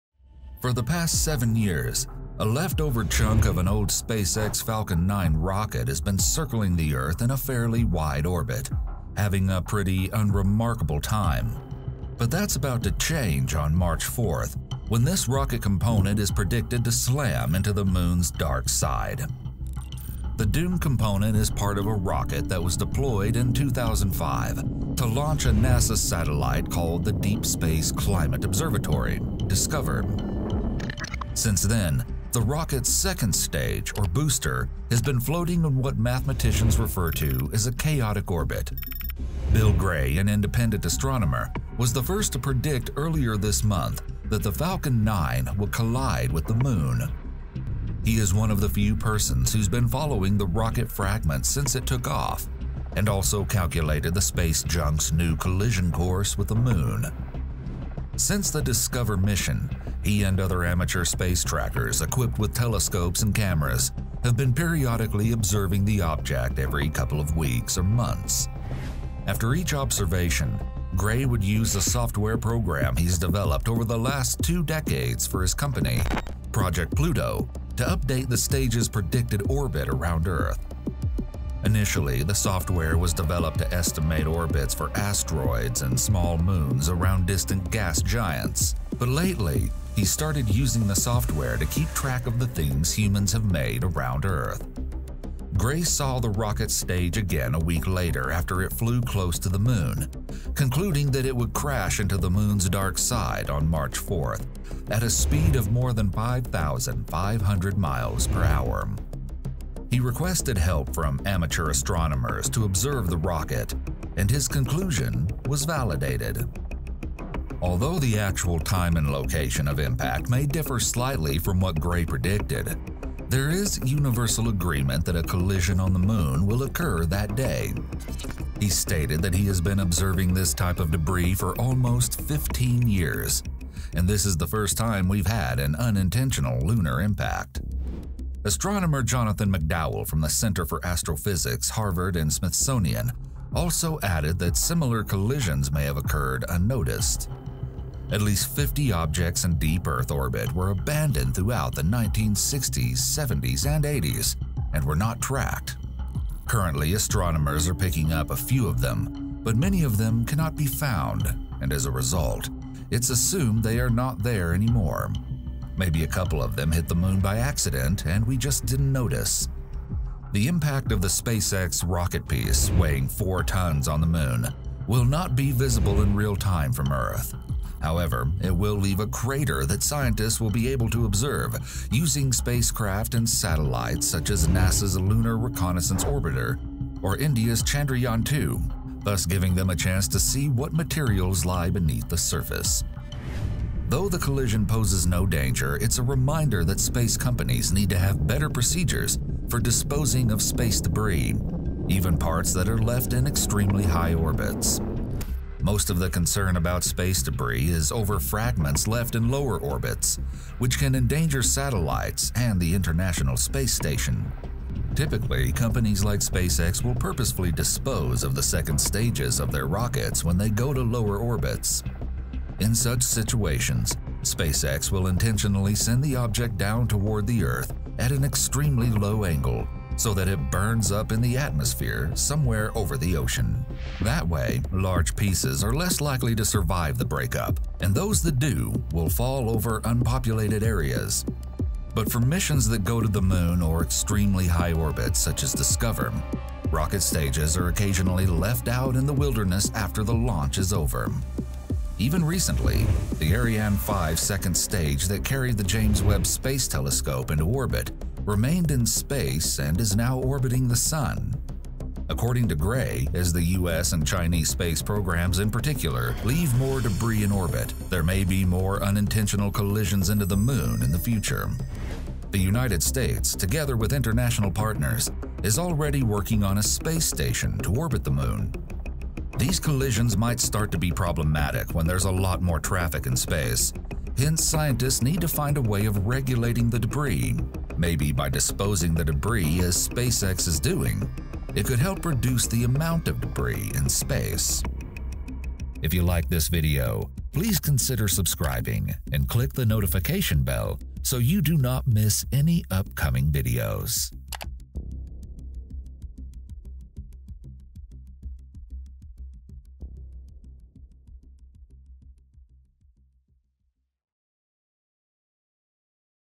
Our Space Garbage Is Now Reaching The Moon
For the past seven years, a leftover chunk of an old SpaceX Falcon 9 rocket has been circling the Earth in a fairly wide orbit, having a pretty unremarkable time. But that’s about to change on March 4th, when this rocket component is predicted to slam into the Moon’s dark side.

Produced & Edited by:
Ardit Bicaj

Written by:
Nicole Amondi

Narrated by:
Russell Archey

iStock.com
NASA's Eyes
NASA/KSC

A big thank you to our lovely members:
Joseph Pacchetti